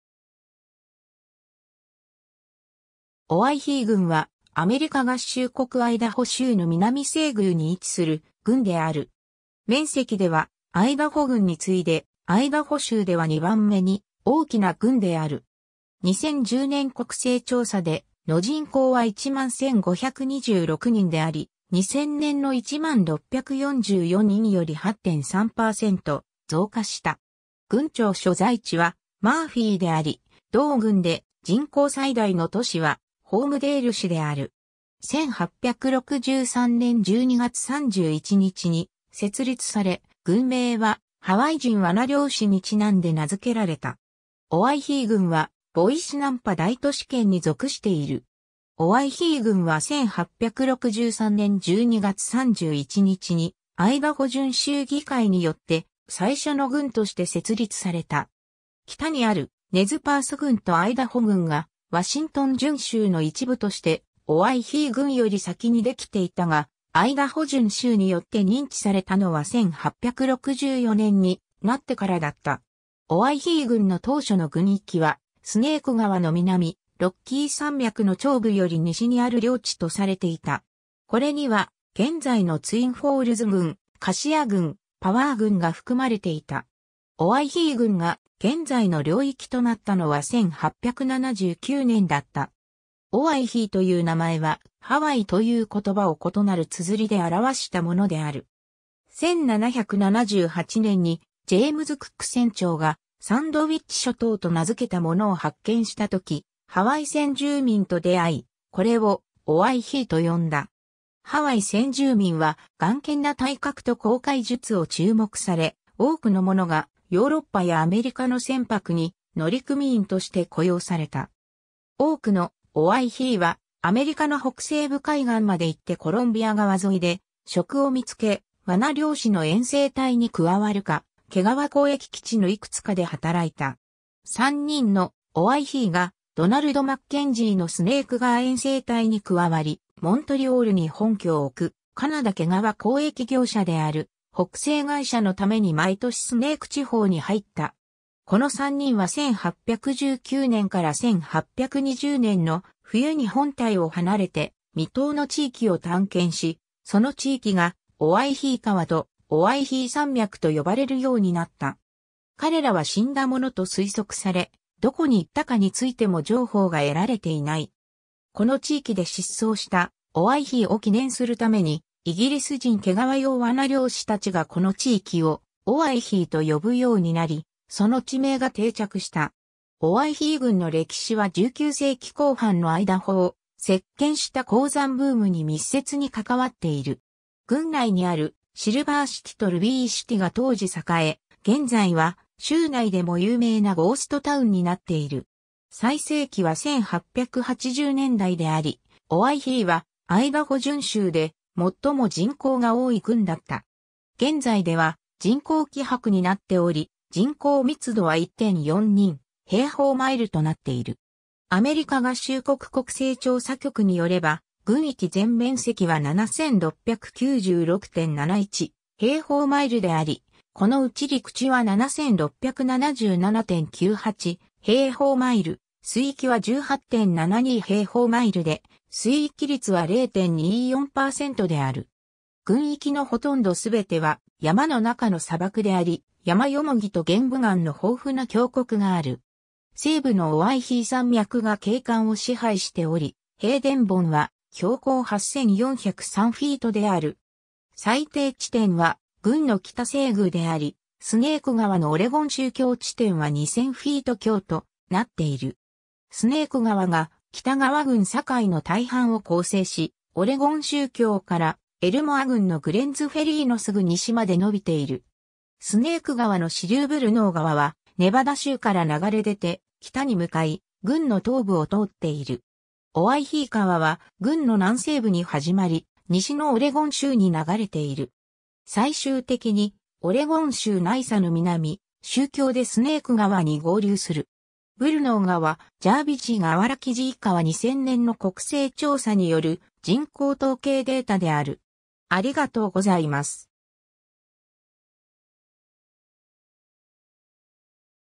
オワイヒー郡 (アイダホ州)
オワイヒー郡（英: Owyhee County）は、アメリカ合衆国アイダホ州の南西隅に位置する郡である。
面積ではアイダホ郡に次いでアイダホ州では2番目に大きな郡である。
2010年国勢調査での人口は11,526人であり、2000年の10,644人より8.3％増加した。
郡庁所在地はマーフィー（人口97人、統計調査指定地域）であり、同郡で人口最大の都市はホームデール市（人口2,633人）である。
1863年12月31日に設立され、郡名はハワイ人罠猟師に因んで名付けられた。
オワイヒー郡はボイシ・ナンパ大都市圏に属している。
 オワイヒー郡は1863年12月31日にアイダホ準州議会によって最初の郡として設立された。
北にあるネズパース郡とアイダホ郡がワシントン準州の一部としてオワイヒー郡より先にできていたが、アイダホ準州によって認知されたのは1864年になってからだった。
 オワイヒー郡の当初の郡域は「スネーク川の南、ロッキー山脈の頂部より西にある領地」とされていた。
これには現在のツインフォールズ郡、カシア郡、パワー郡が含まれていた。
オワイヒー郡が現在の領域となったのは1879年だった。
 オワイヒーという名前は「ハワイ」（"Hawaii"）という言葉を異なる綴りで表したものである。
1778年にジェームズ・クック船長がサンドウィッチ諸島（現在のハワイ諸島）と名付けたものを発見したとき、ハワイ先住民と出遭い、これを「オワイヒー」と呼んだ。
ハワイ先住民は頑健な体格と航海術を注目され、多くの者がヨーロッパやアメリカの船舶に乗組員として雇用された。
多くの「オワイヒー」はアメリカの北西部海岸まで行ってコロンビア川沿いで職を見つけ、罠猟師の遠征隊に加わるか、毛皮交易基地の幾つかで働いた。
 3人の「オワイヒー」がドナルド・マッケンジーのスネーク川遠征隊に加わり、モントリオールに本拠を置くカナダ毛皮交易業者である北西会社のために毎年スネーク地方に入った。
この3人は1819年から1820年の冬に本隊を離れて未踏の地域を探検し、その地域がオワイヒー川とオワイヒー山脈と呼ばれるようになった。
彼らは死んだものと推測され、どこに行ったかについても情報が得られていない。
この地域で失踪した「オワイヒー」を記念するために、イギリス人毛皮用罠猟師達がこの地域をオワイヒーと呼ぶようになり、その地名が定着した。
 オワイヒー郡の歴史は19世紀後半のアイダホを席捲した鉱山ブームに密接に関わっている。
郡内にあるシルバーシティとルビーシティが当時栄え、現在は州内でも有名なゴーストタウンになっている。
最盛期は1880年代であり、オワイヒーはアイダホ準州で最も人口が多い郡だった。
現在では人口希薄になっており、人口密度は1.4人/平方マイル (0.5 人/km²) となっている。
 郡域のほとんどすべては山の中の砂漠であり、ヤマヨモギと玄武岩の豊富な峡谷がある。
西部のオワイヒー山脈が景観を支配しており、ヘイデン峰は標高8,403フィート (2,561 m) である。
最低地点は郡の北西隅であり、スネーク川のオレゴン州境地点は2,000フィート (610 m) 強となっている。
スネーク川が北側郡境の大半を構成し、オレゴン州境からエルモア郡のグレンズフェリーのすぐ西まで伸びている。
スネーク川の支流ブルノー川はネバダ州から流れ出て北に向かい、郡の東部を通っている。
オワイヒー川は郡の南西部に始まり、西のオレゴン州に流れている。
最終的にオレゴン州ナイサの南、州境でスネーク川に合流する。
 ブルノー川・ジャービッジ川原生地 以下は2000年の国勢調査による人口統計データである。